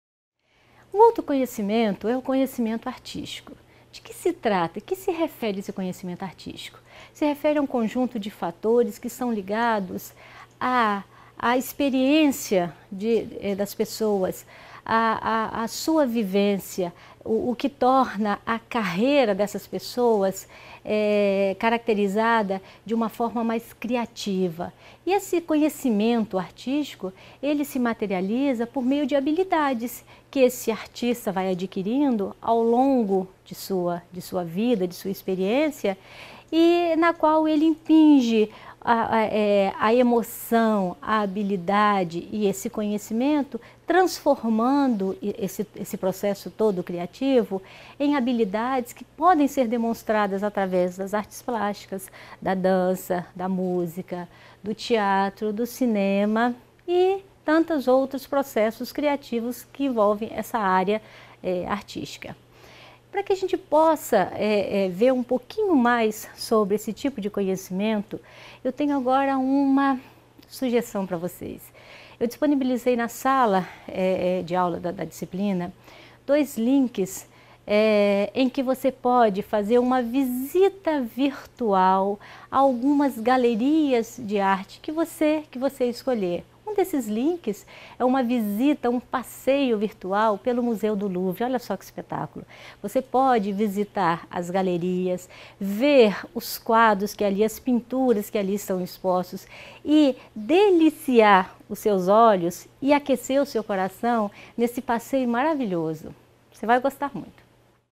Metodologia de pesquisa - Modulo 1 - Conhecimento Artístico [Libras]
Curso: Pós - Graduação Em Tecnologias Educacionais
Disciplina: Metodologia de pesquisa
Professor: Esther Ortlieb Faria de Almeida
Semana: Módulo 1